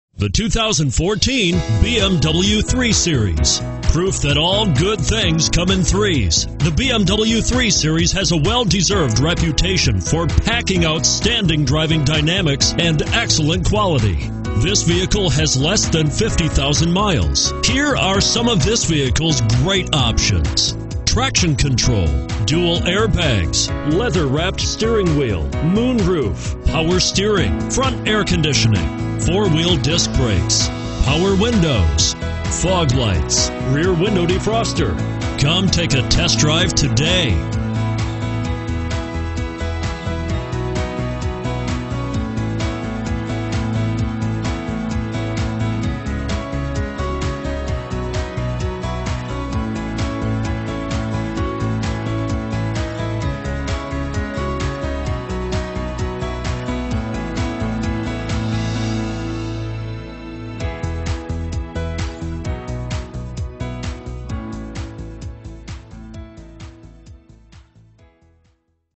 2014 BMW 3 Series Schererville, Chicago, Gary, Lansing P3359
2014 BMW 3 Series 320i xDrive

One Owner!!, LOCAL TRADE, and SUNROOF MOONROOF. BMW Certified and AWD. Get serious momentum going. Ambient noise is held at bay. brbrThere are used cars, and then there are cars like this well-taken care of 2014 BMW 3 Series.  This luxury vehicle has it all, from a posh interior to a wealth of wonderful amenities.  If you want a creampuff with style, this is it. Sit in the land of milk and honey. BMW Certified Pre-Owned means you not only get the reassurance of up to a 6yr/100,000 mile limited warranty, but also a multipoint inspection/reconditioning, 24/7 roadside assistance, trip-interruption services, and a vehicle history report.